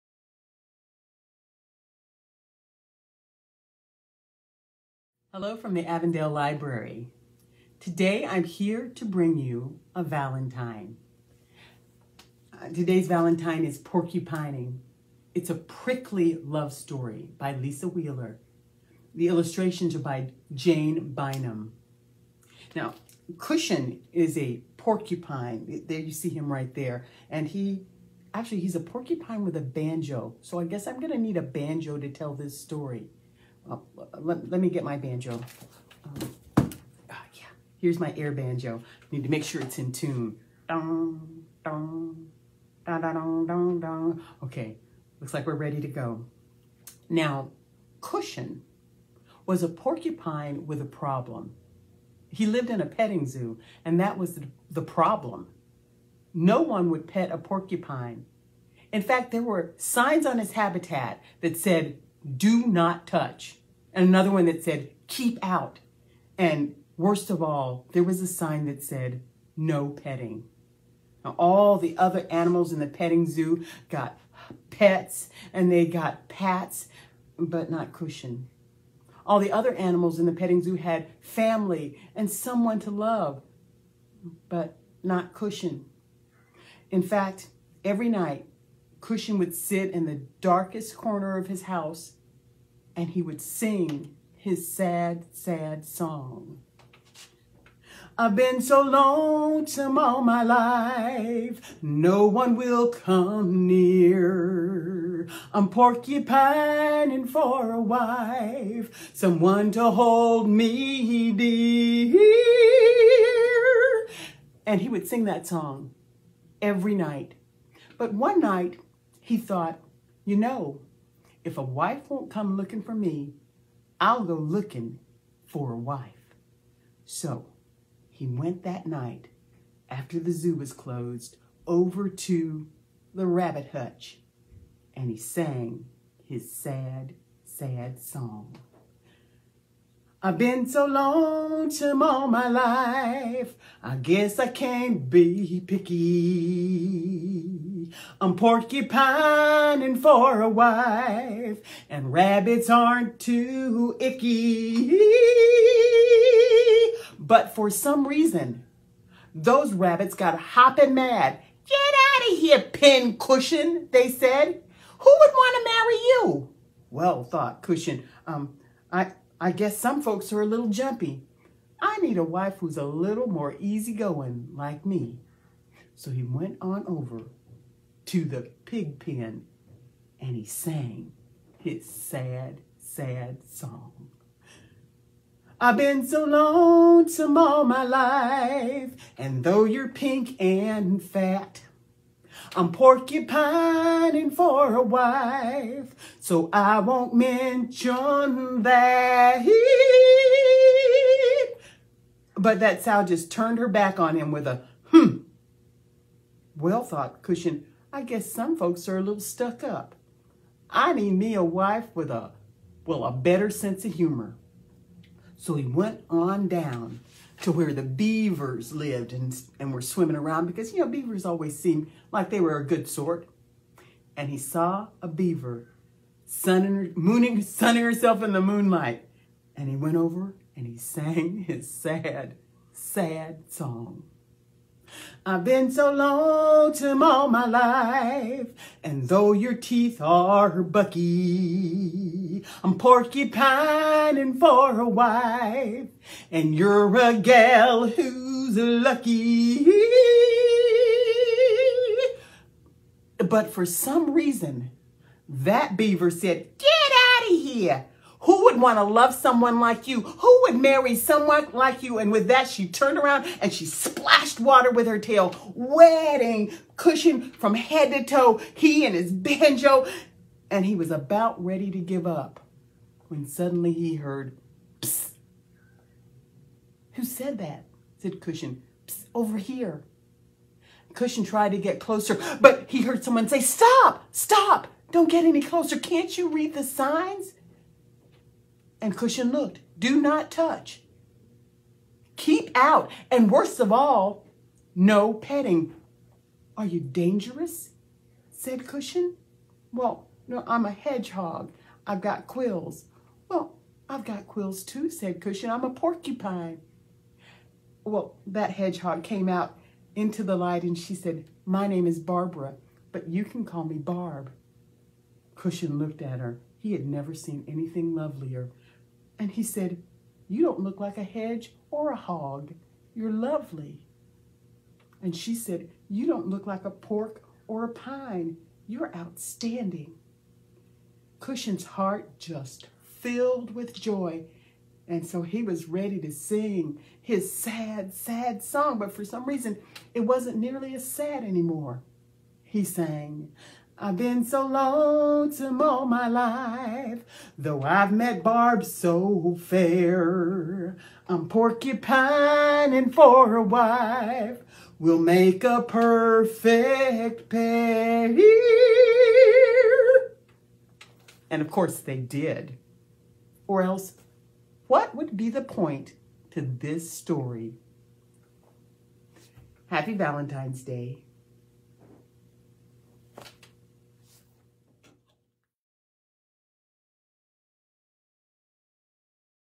Porcupining: A Love Story
Love isn’t always easy to find, especially if you’re a porcupine! Enjoy this heartwarming story from Ms. Cas on today’s Tales and Tidbits.